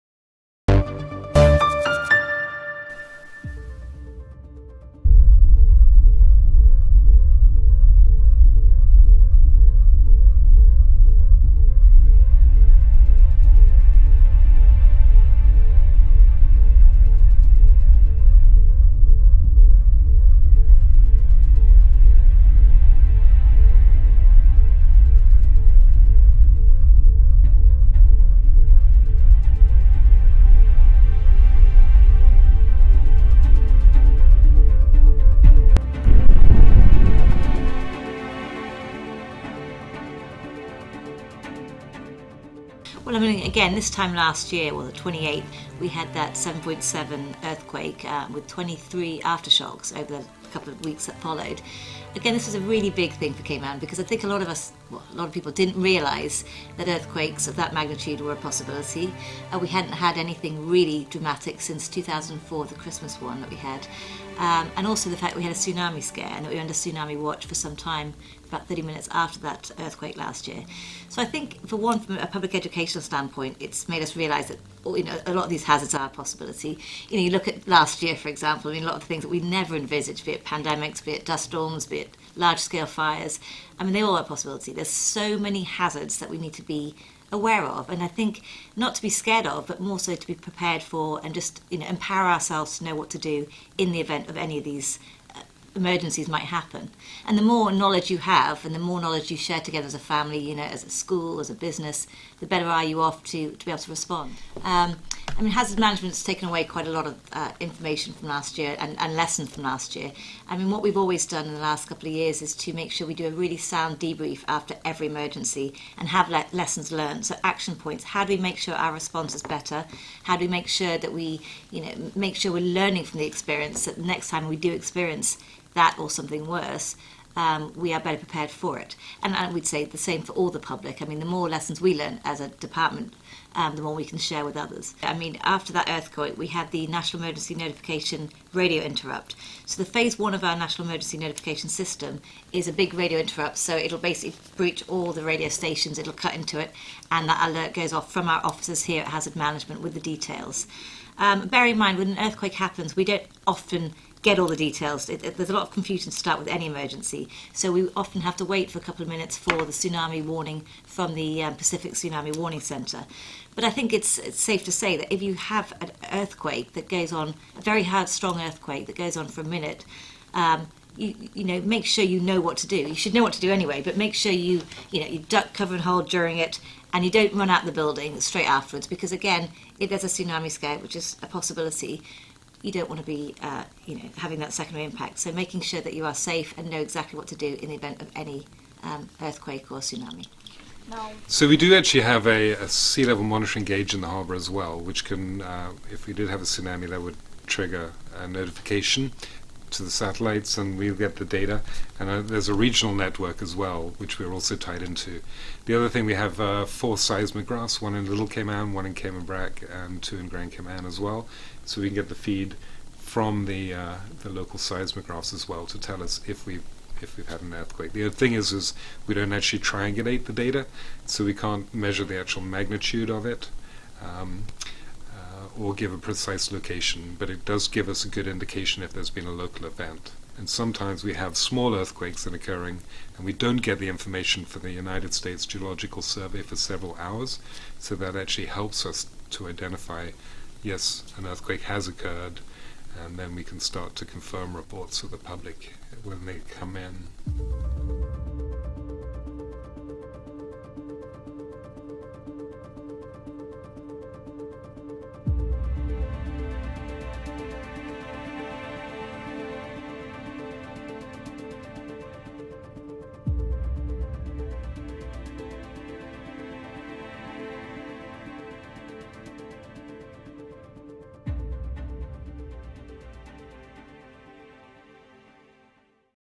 7 7 magnitude earthquake remembered
🔴 Video | Midway through Tuesday, 28 Jan. 2020, the Cayman Islands was rocked by a 7.7 magnitude earthquake. The quake, which originated at a depth of six miles and lasted for about two minutes, was felt in Cayman, Jamaica and Cuba, according to the United States Geological Survey.